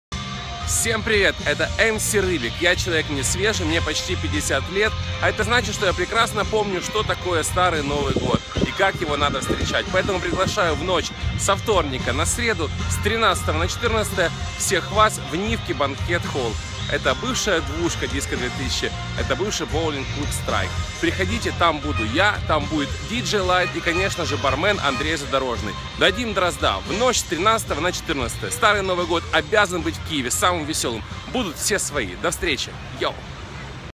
МС Рыбик приглашает на Старый Новый Год!
13 января, Киев, Nivki банкет-холл, DJ Light!